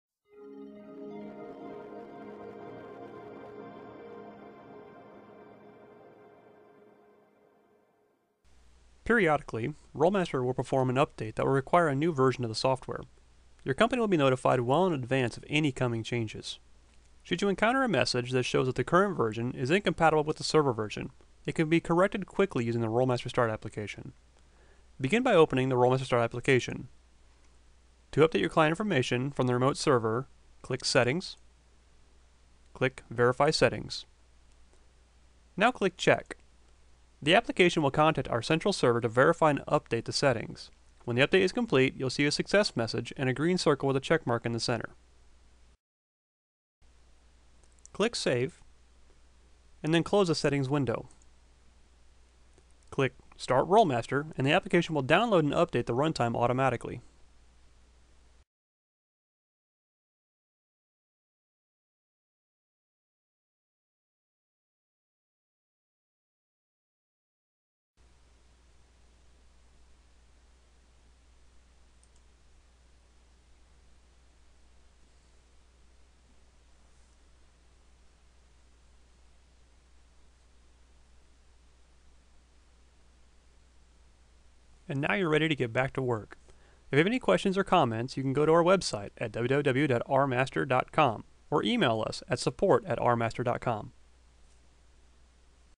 RM Start Automatic Updates
Help automate your client updates using the RM Start application